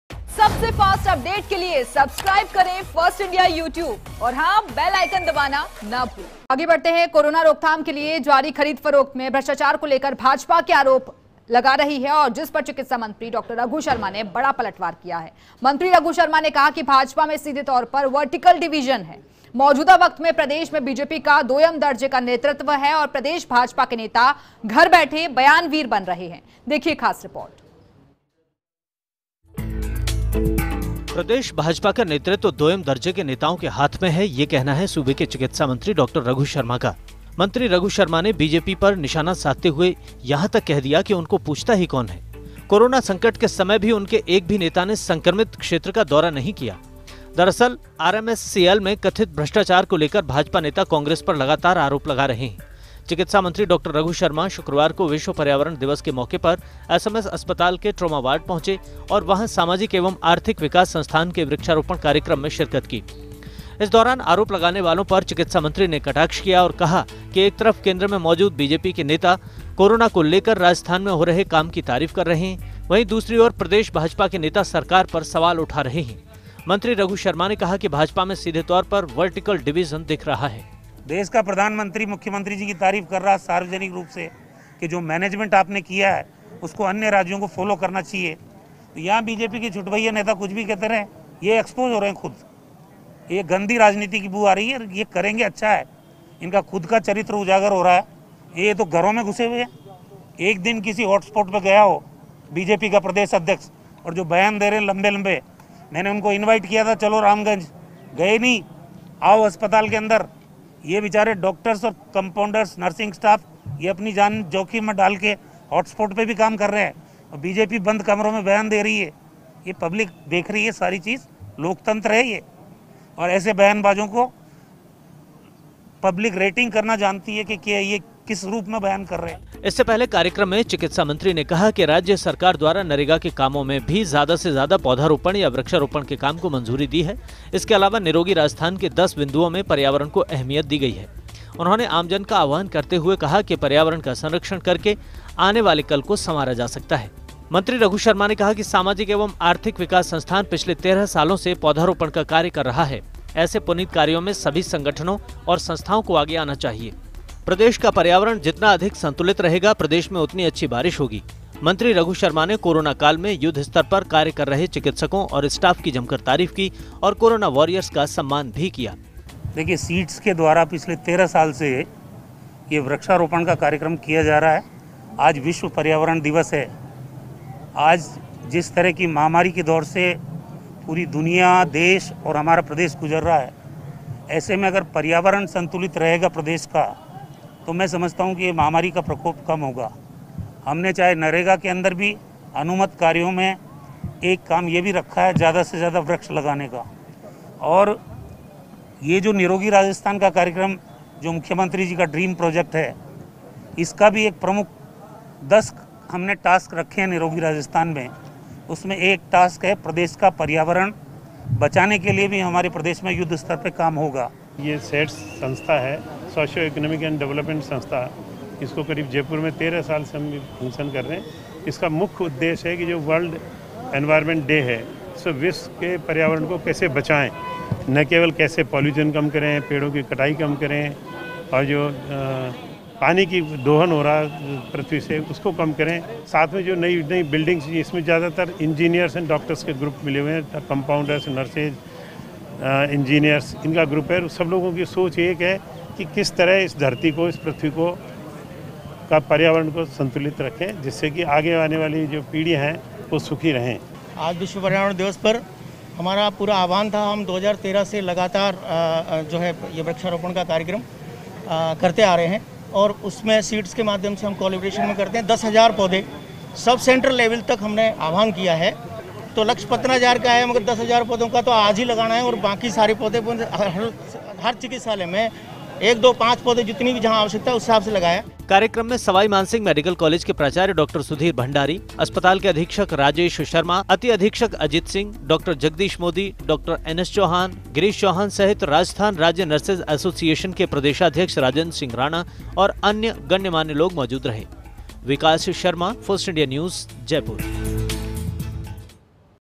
मंत्री Raghu Sharma ने BJP पर किया पलटवार, कहा-प्रदेश BJP का नेतृत्व दोयम दर्जे के नेताओं के हाथ में
About this Video:
Minister Raghu Sharma hit back at BJP, said - State BJP leadership in the hands of second-rate leaders